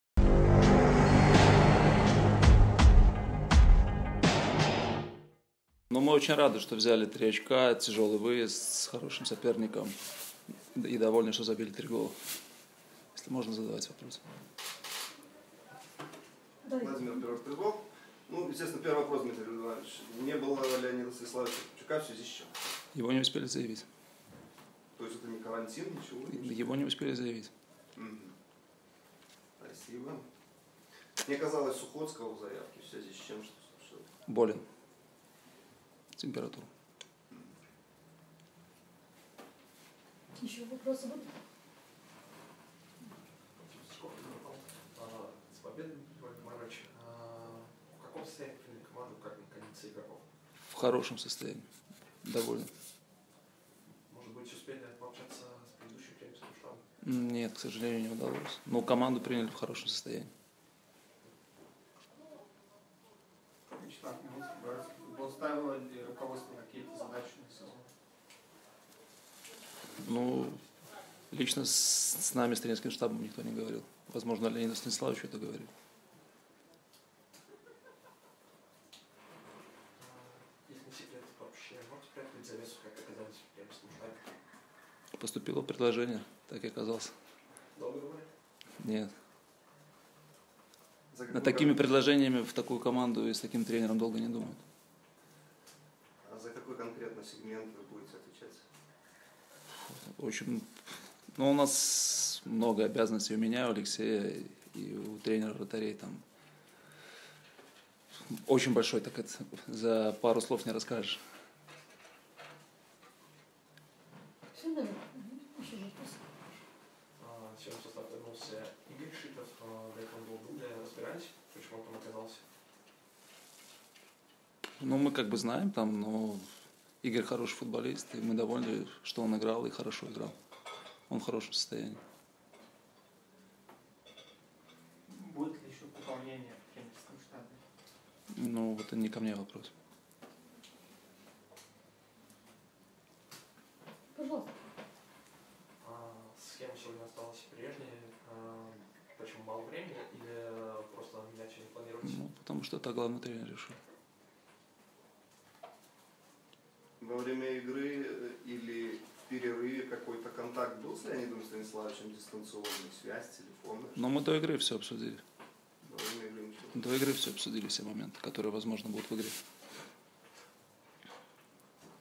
Дмитрий Ленцевич: "Команду приняли в хорошем состоянии"
Пресс-конференция после победного матча 6-го тура.
Вопросы журналистов в закрепленном комментарии под видео
Мы в сети: